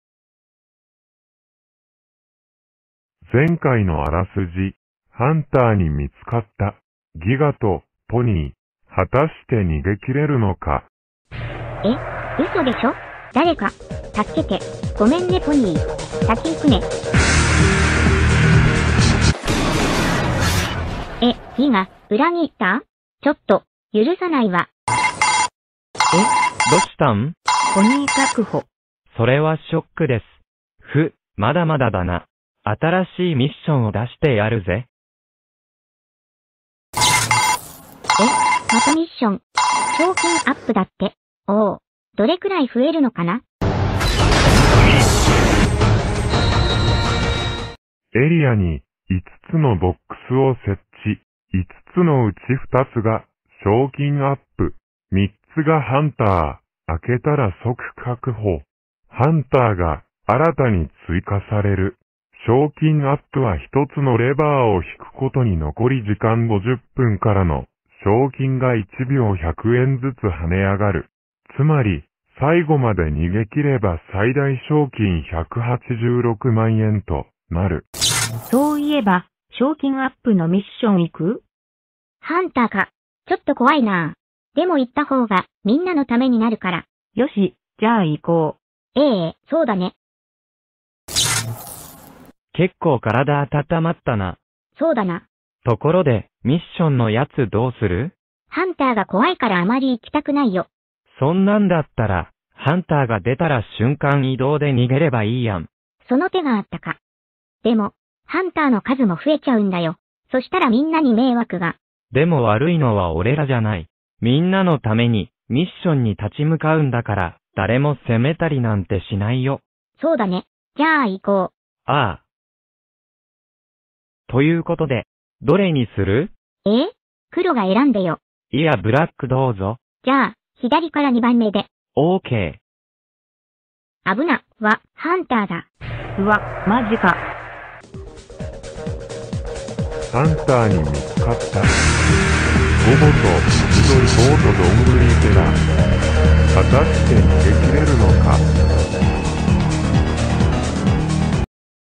アニメ ～逃走中～ part17
～逃走中～
制限時間は105分　1秒につき200円　賞金126万円　途中参加はありですが、始めた時に0から増えていくので、始めからやった人よりも賞金は少ないです。